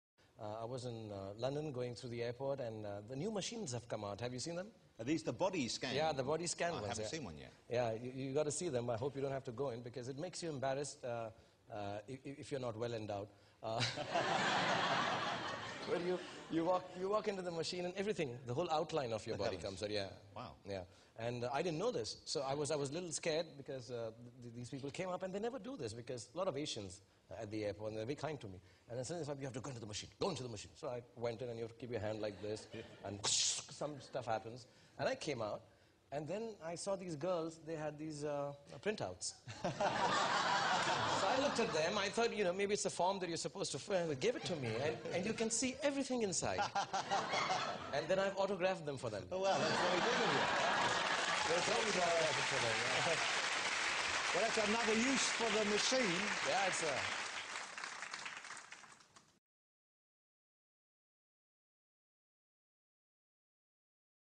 SRK on airport scanners - BBC interview with Jonathan Ross
SRK talks about airport scanners with BBC Jonathan Ross